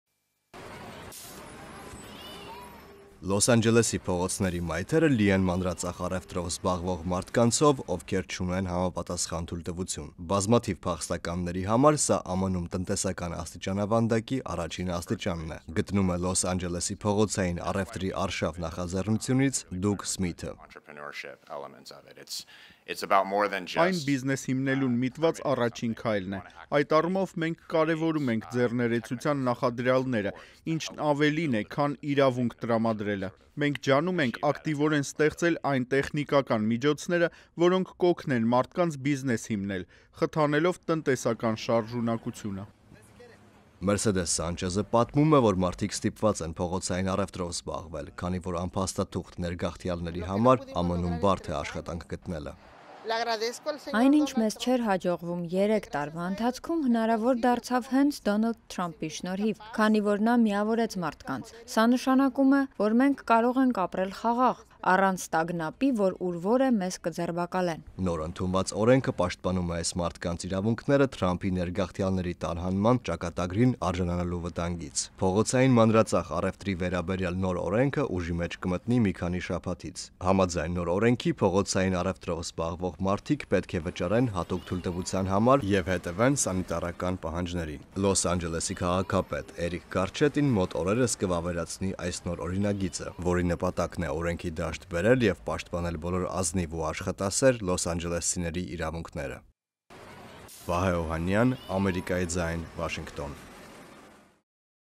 Լոս Անջելես-ը ապաքրեականացնում է փողոցային բիզնեսը
Ամերիկյան քաղաքային կյանքի մաս կազմող փողոցային մանրածախ առևտրով զբաղվող մարդկանցից շատերը անփաստաթուղթ ներգաղթյալներ են: Լոս Անջելեսում փողոցային մանրածախ առևտրի կետերի վերաբերյալ նոր օրենքը ուժի մեջ կմտնի մի քանի շաբաթից՝ օրենքի դաշտ բերելով բոլոր ազնիվ ու աշխատասեր մարդկանց: